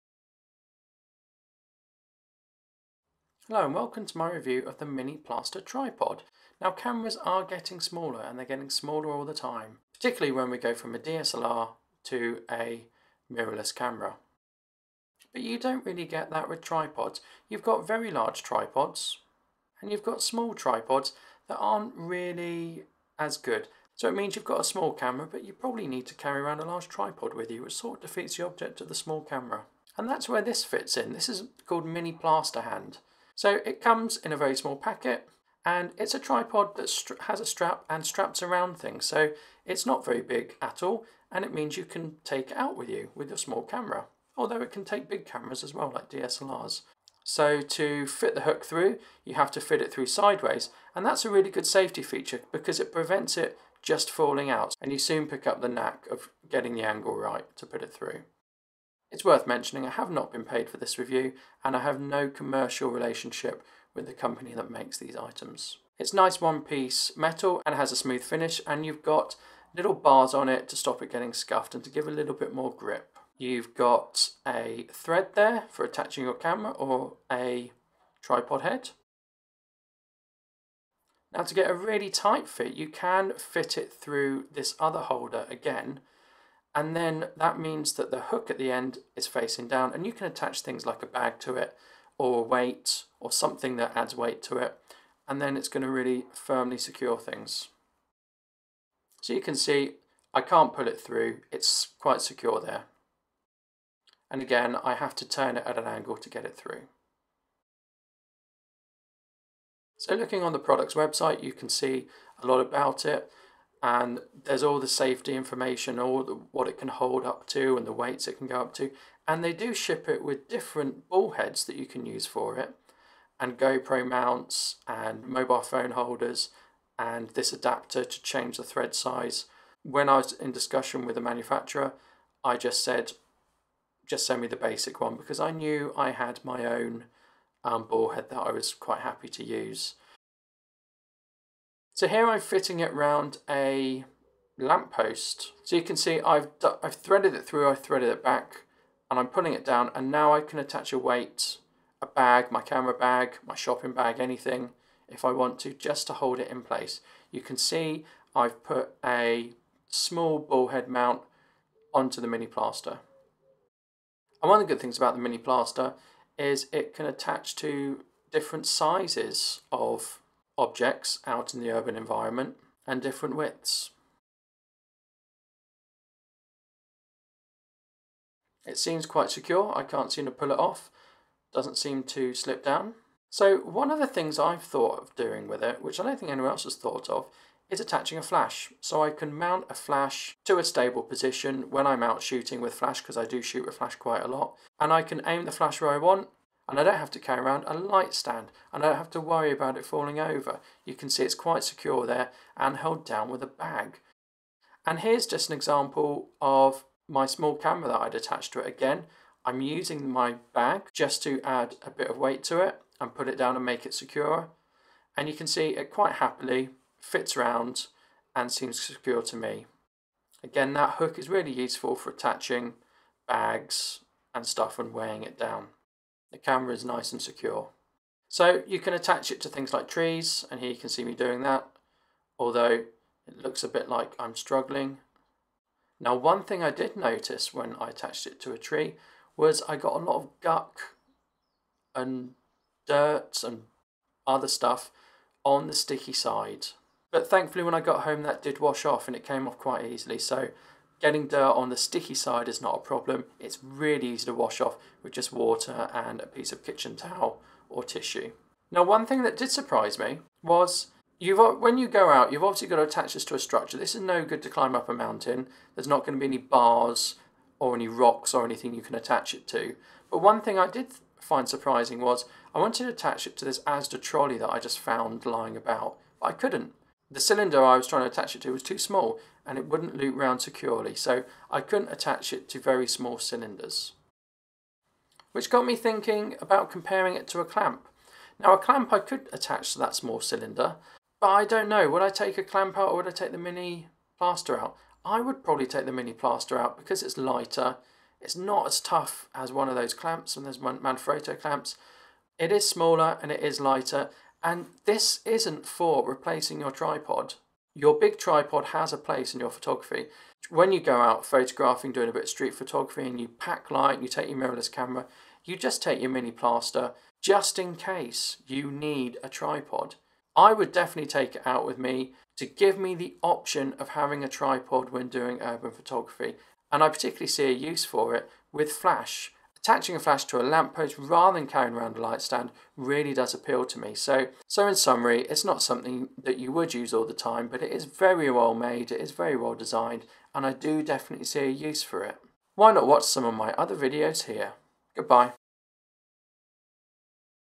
Mini tripod review: The Mini Plaster hand
This is my review of the Mini Plaster Hand micro tripod. I was not paid for this review and remain impartial. Find more details about the Mini Plaster Hand